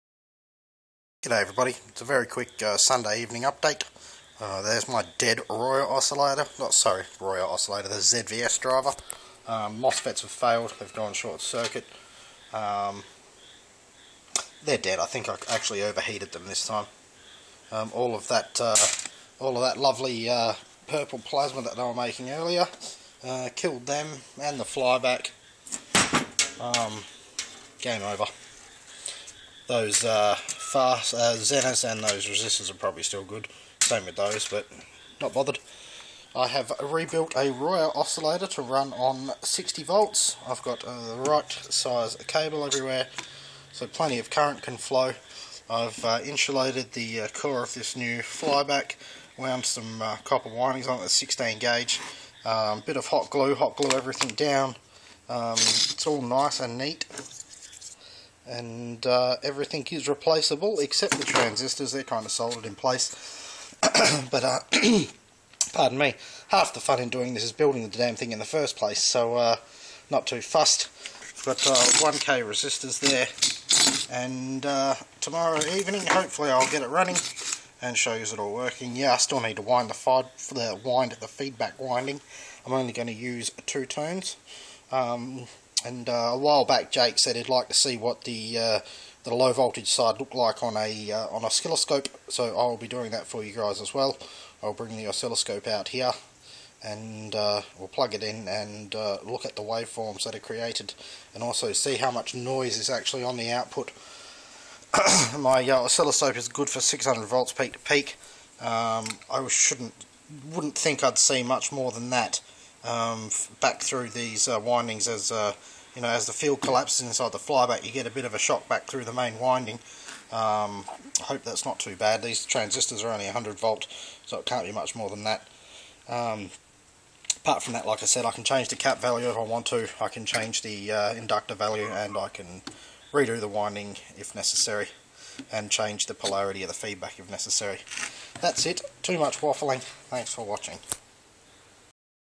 Sunday Stress Pt. II :- Royer Oscillator Flyback Driver ReVisit ReBuild
As the title says! nice neat build onto one heat sink for small size and portability!
im waiting for my 20 IGBTs to show up from china ! until then i play with royer because i have a few TIP35c spare...;p but can still have some properly filthy fun with this :-)
Setup the "Device" for 60 volts this time ( raymond ;-) )
everything should be right :-)
Might add some 15 volt zeners for base protection. i dont know if this would work like it does with a ZVS.. but thats experimentation for you!